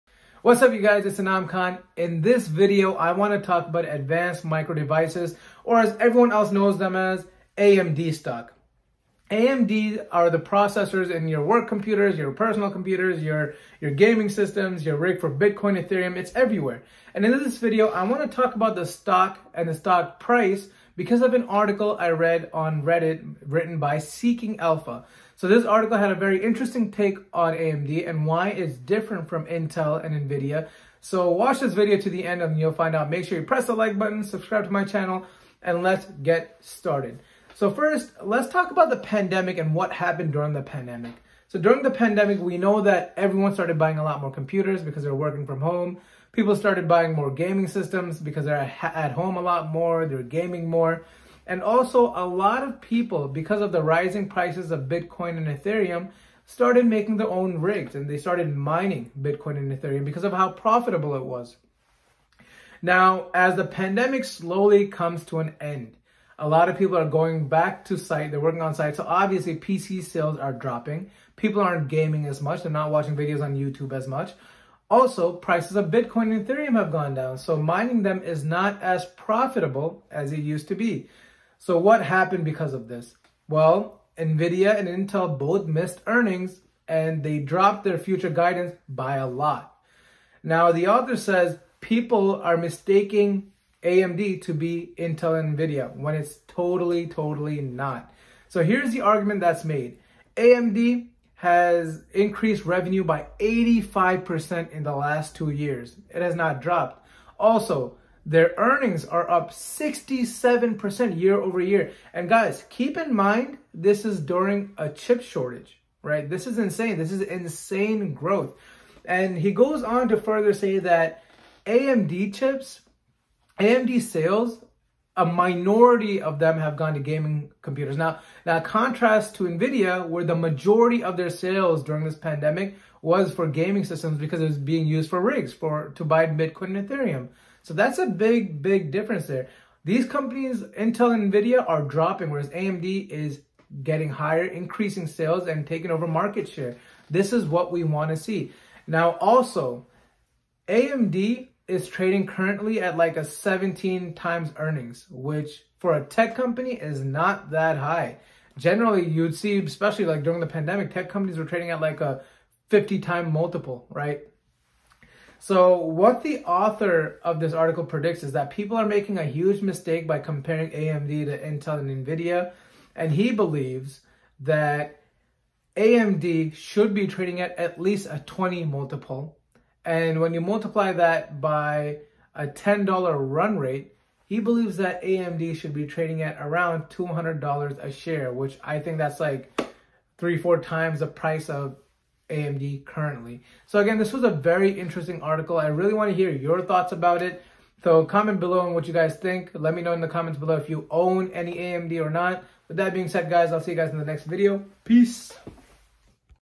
How high can AMD stock go?
Advanced Micro Devices (AMD stock) has had a lot of hype lately. I came across an article by Seeking Alpha that makes the argument that AMD is different from Intel and NVDA stock.

During the pandemic, computers were being left and right due to everyone working from home. Demand for processing chips went really high. In addition, Bitcoin and Ethereum prices skyrocketed, so people started mining it. To mine Bitcoin and Ethereum, they used AMD and NVDA processors.

As the pandemic comes to an end, demand is starting to dwindle down. NVDA and Intel missed earnings and dropped future forecasts. People think AMD will go through something similar, watch the video to find out why AMD is different.